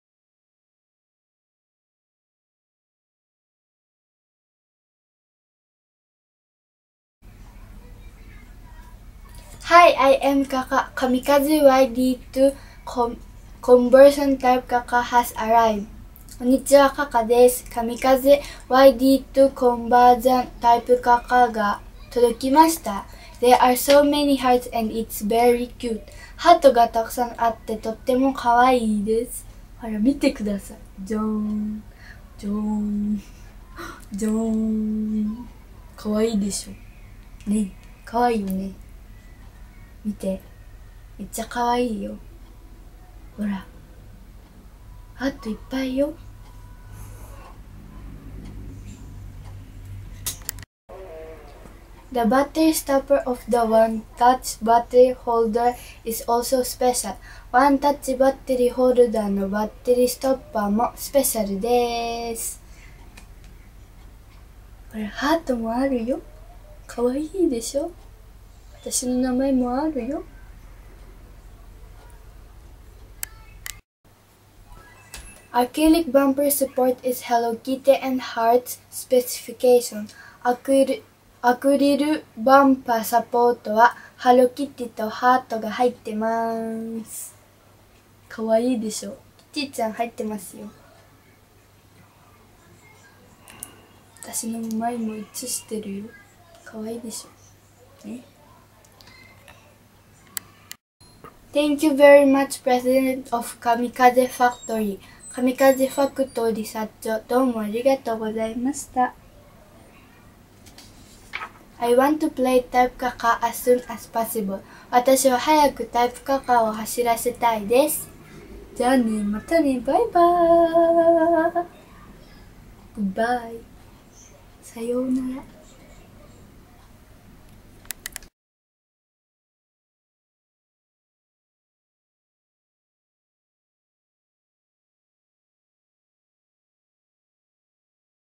KAMIKAZE YD2 Comversion Type CaCa Arrived. Lady CaCa's Wonderful R/C Life Vol,14
Kamikaze comversion produced by CaCa has arrived.
There are many hearts.
CaCaプロデュースの、神風コンバージョンが、届きました。
ハートが、沢山入ってます